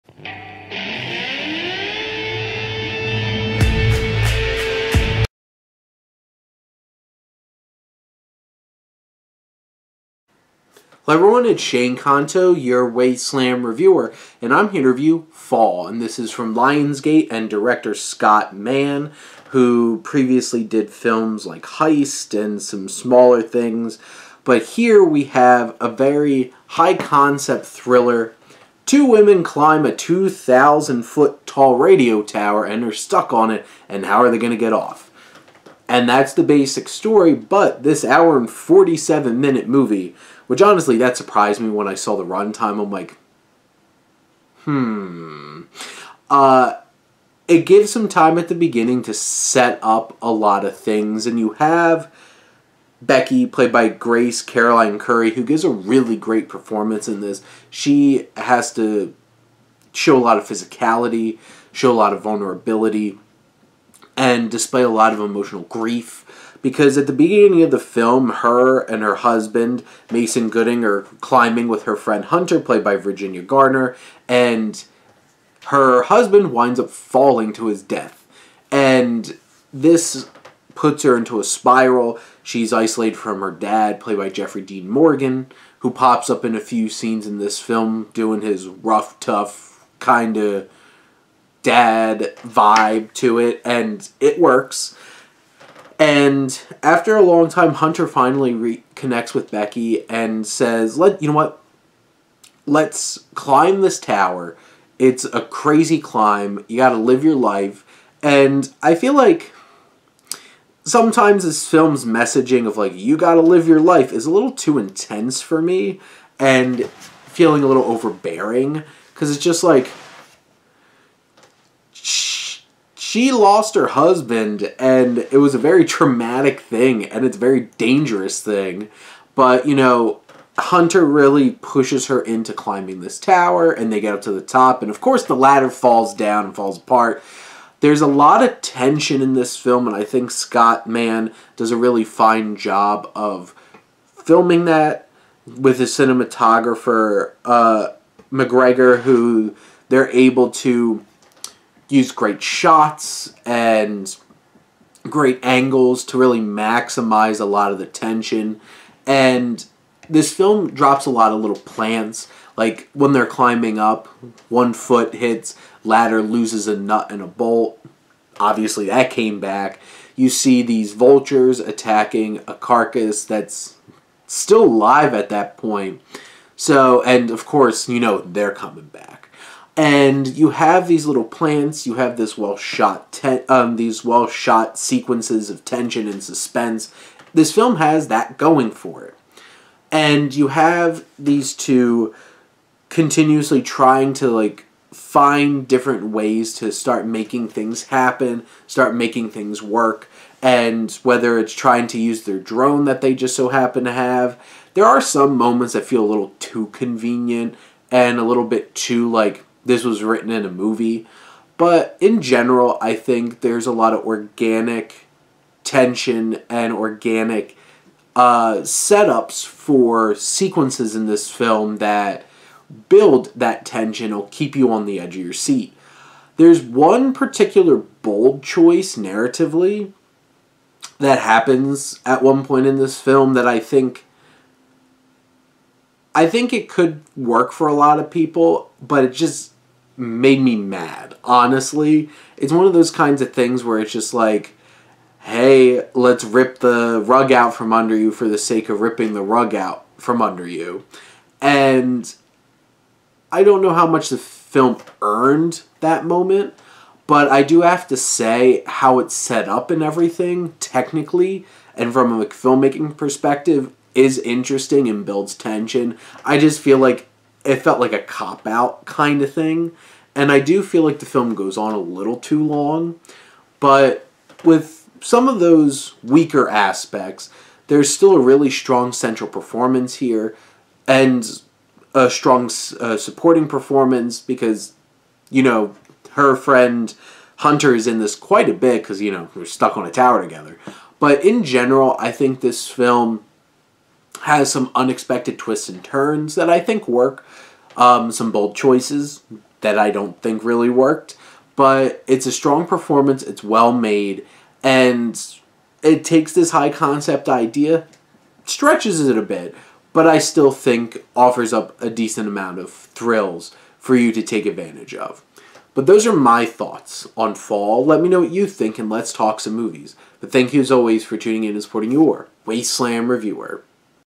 Fall (2022) - Wasteland Film Review
My Wasteland Review of Fall
Theaters August 12th
Synopsis: Best friends Becky and Hunter find themselves at the top of a 2,000-foot radio tower.